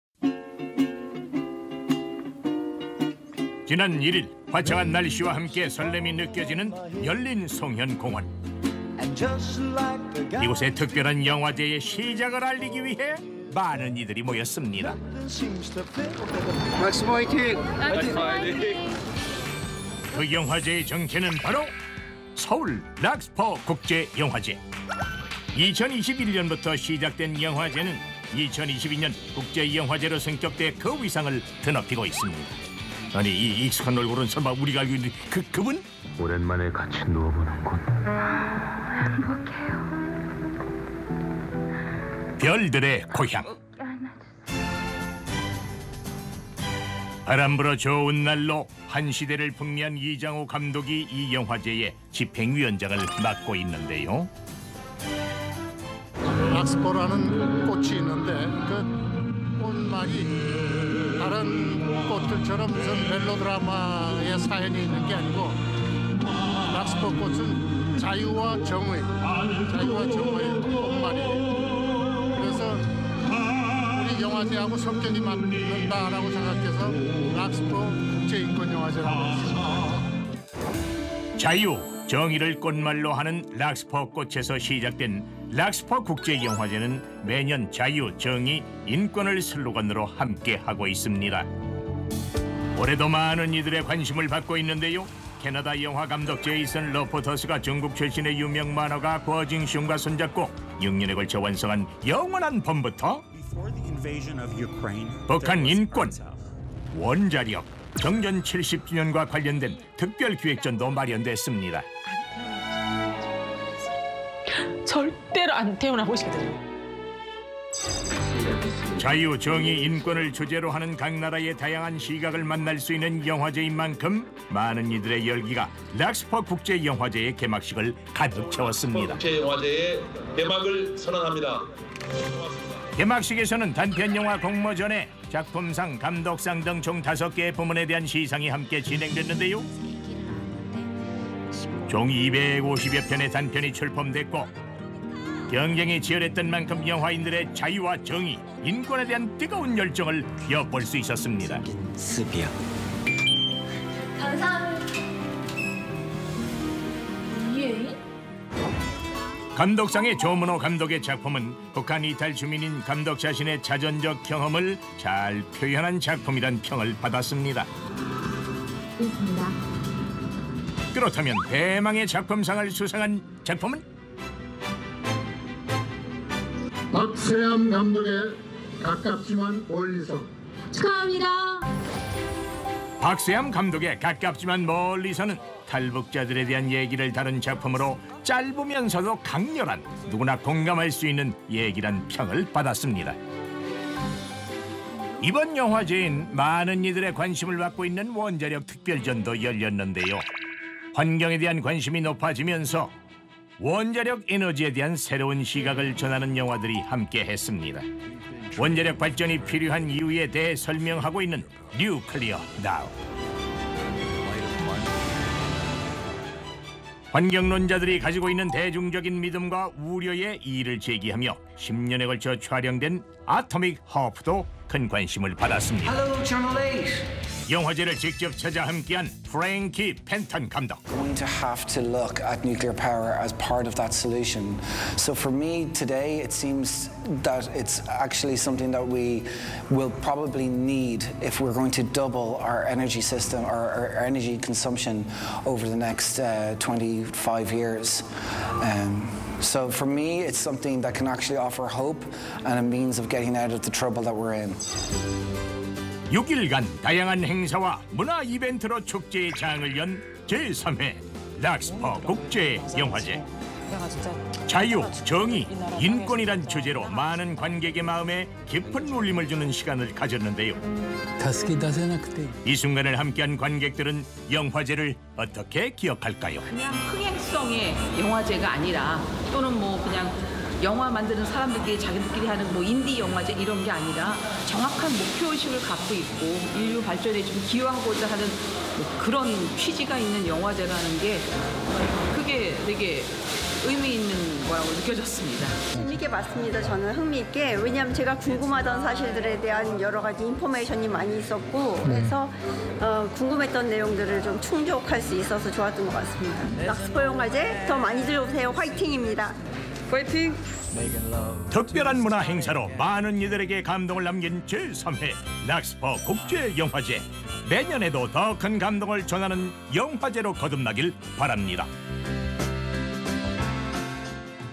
탈북자 이야기부터 원자력까지! 자유,정의,인권을 위한 제3회 서울 락스퍼국제영화제 개최 | 행복한 아침 1119 회
특별한 문화 현장!
제3회 서울 락스퍼국제영화제